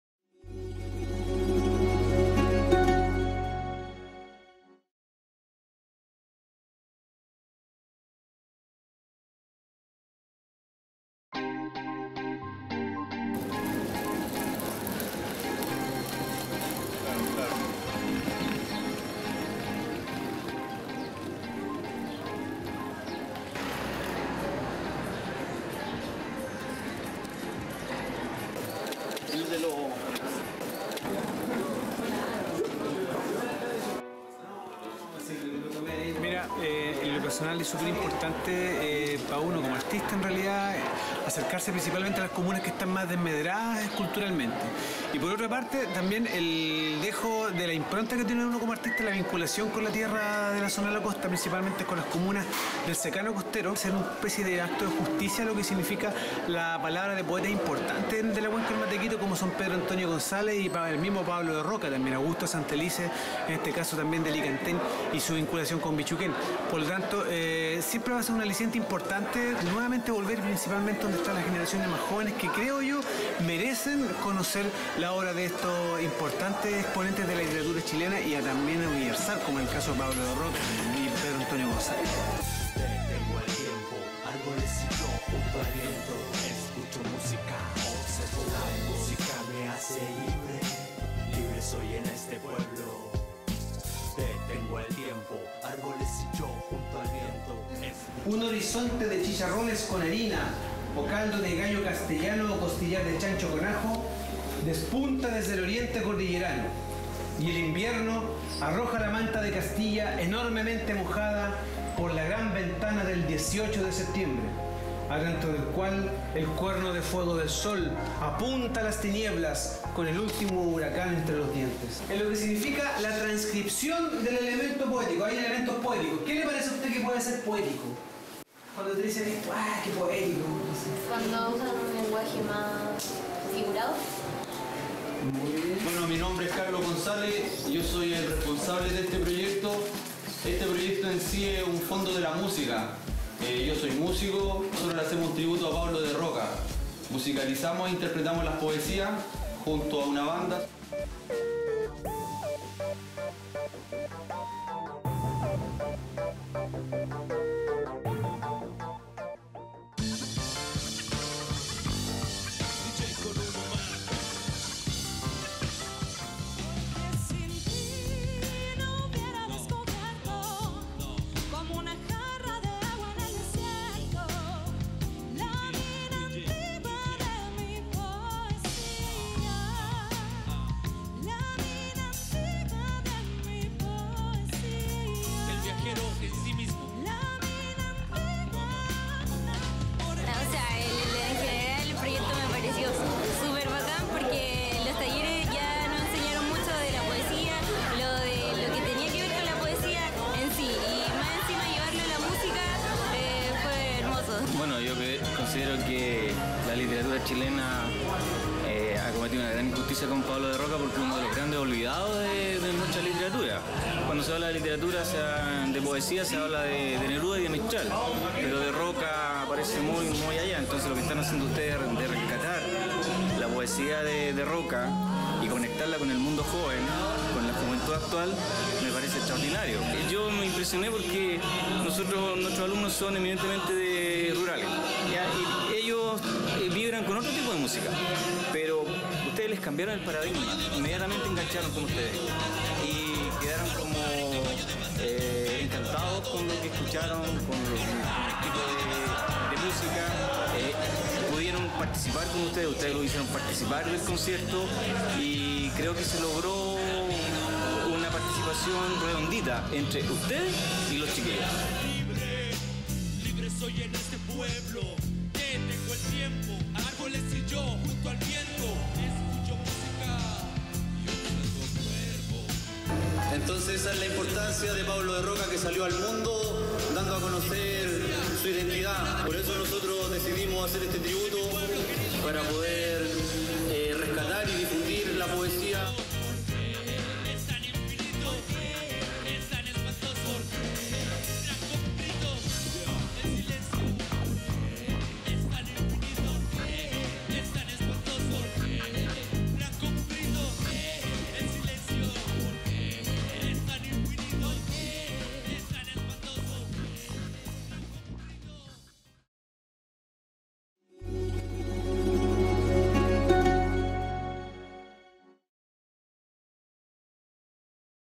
VERSO DE ROKHA EN CUREPTO - TALLER DE POESÍA Y CONCIERTO
Fondo de la Música 2017- Se realizó un Ciclo de 4 Conciertos.
El Gimnasio de Curepto, se vivió una gran jornada de Música y Poesía. Un público alegre y con mucho entusiasmo y energia, disfrutaron de las melodías contagiosas del Hip Hop de VERSO DE ROKHA.